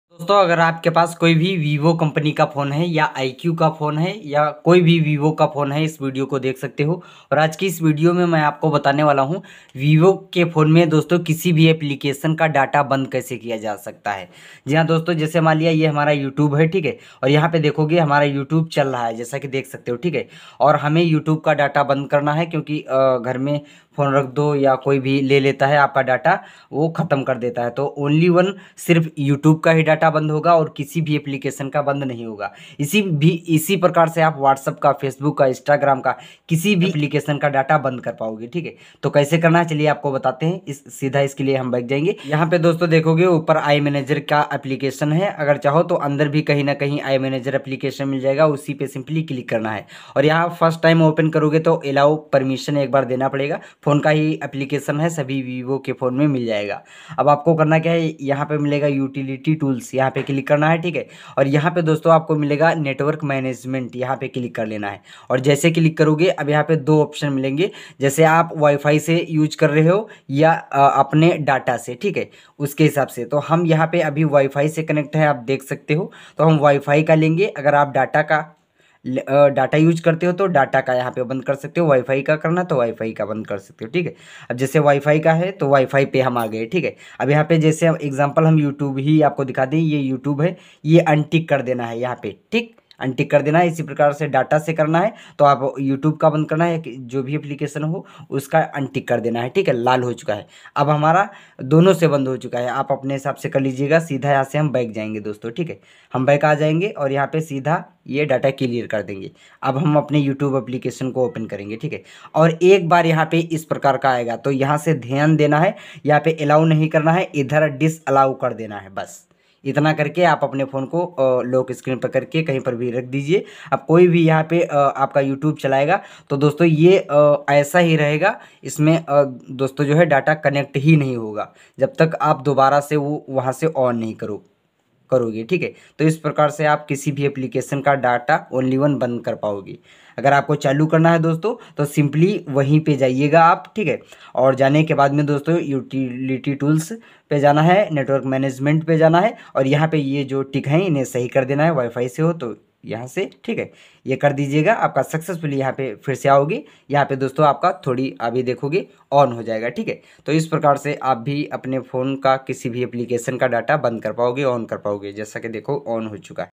vivo mobile me kisi bhi app ka Data dand kaise kare / kisi bhi app ka data off kaise kare
Hello dosto aaj ki is video mein humne aapko bataya vivo mobile me kisi bhi app ka internet band kaise karen